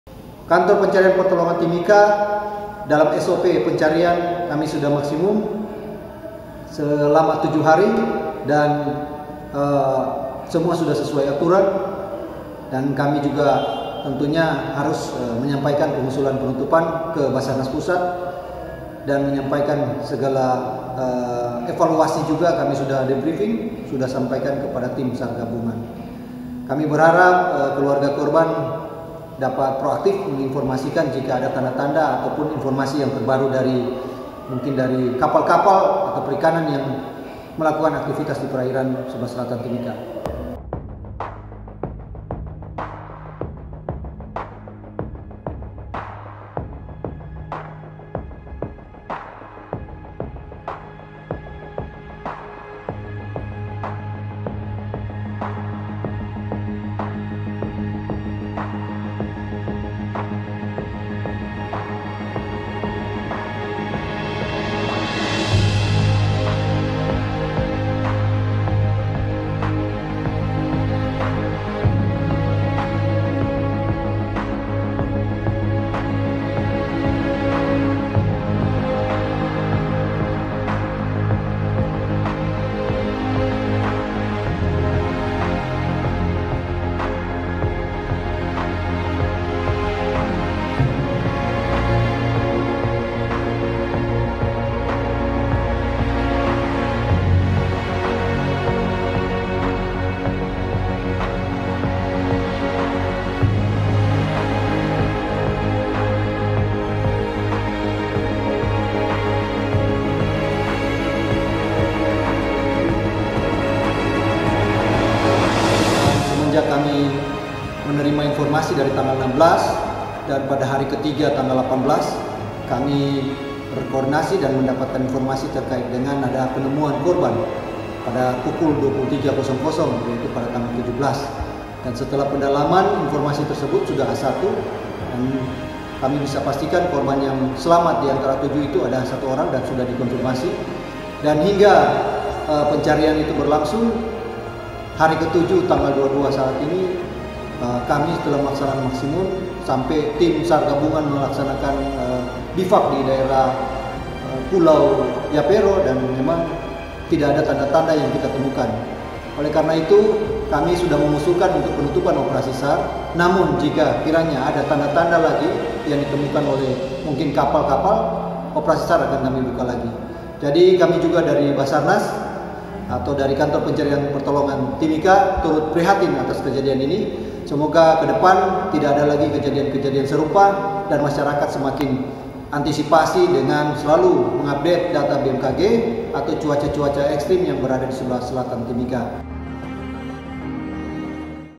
Operasi SAR Pencarian KM Uty Star Ditutup, 6 Korban Belum Ditemukan
Kantor Pencarian dan Pertolongan Timika, Papua penutupan operasi SAR pencarian terhadap 7 penumpang Kapal Motor (KM) Uty Star yang dilaporkan terggelam pada 16 Februari 2021 di perairan sekitar pulau Yapero, Distrik Mimika Timur Jauh, Mimika, Papua.

Pencarian KM Uty Star dilakukan selama tujuh hari sejak tanggal 16 sampai 22 Februari 2021. Sesuai SOP Badan Nasional Pencarian dan Pertolongan, akhirnya operasi SAR terpaksa dihentikan.

Aktifkan juga loncengnya supaya dapat notifikasi video terbaru dari Seputar Papua.